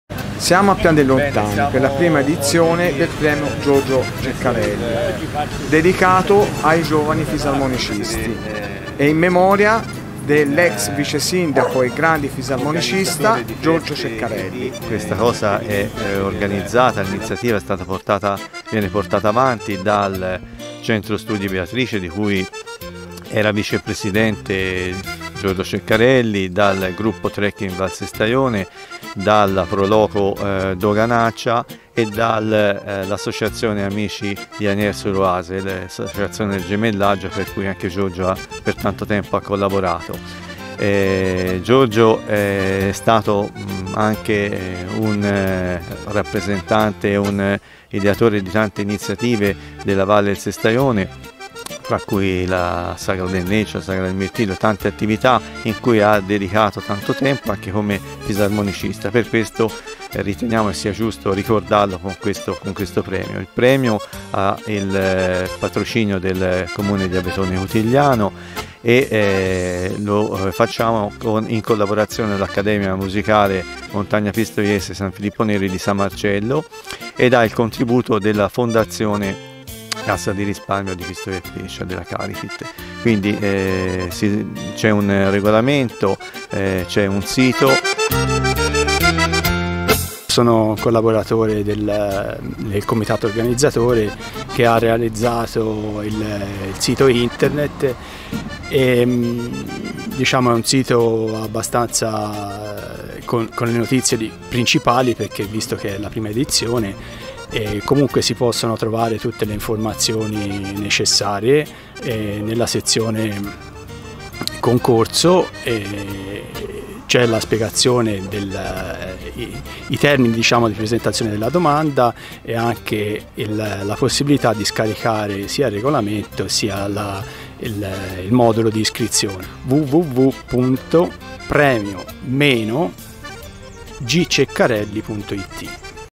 Un premio per i giovani fisarmonicisti
Presentato un nuovo premio, dedicato alla memoria di Giorgio Ceccarelli, rivolto ai giovani fisarmonicisti.